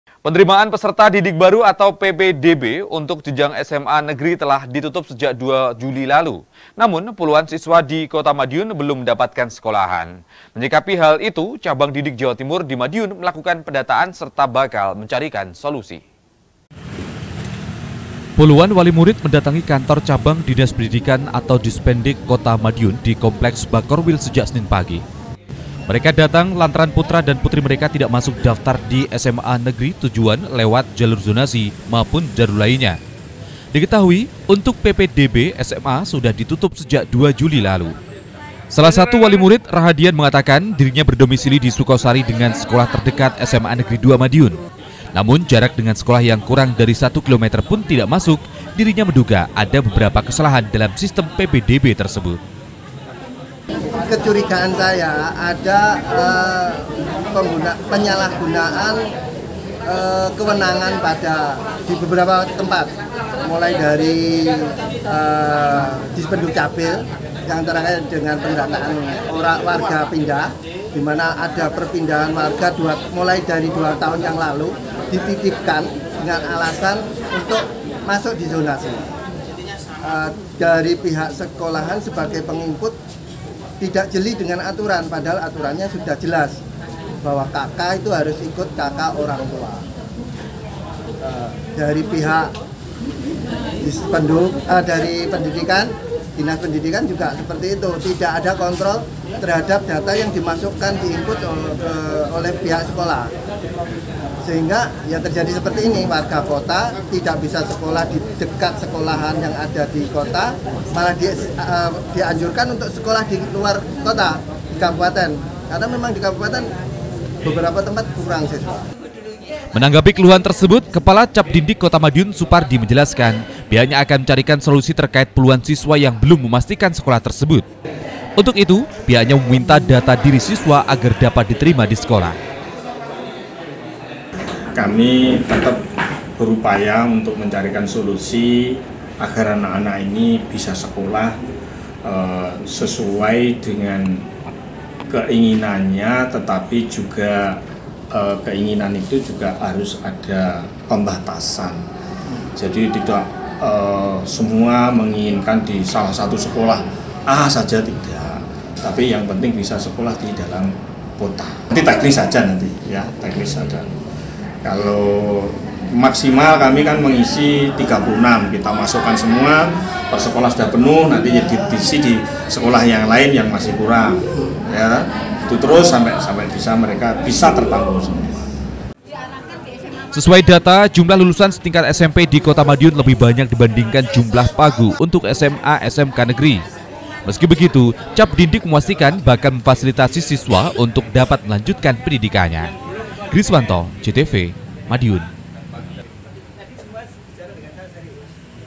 KOTA MADIUN - Anak Belum Dapat Sekolah, Puluhan Wali Murid Datangi CABDINDIK
Penerimaan peserta didik baru (ppdb) untuk jenjang sma negeri telah ditutup sejak 2 juli lalu. Namun puluhan siswa di kota madiun belum mendapatkan sekolah. Menyikapi hal itu cabang dindik jatim di madiun melakukan pendataan serta bakal mencarikan solusi.
Office : jl. Sarimulya no 47 Rejomulyo, Kartoharjo, Kota Madiun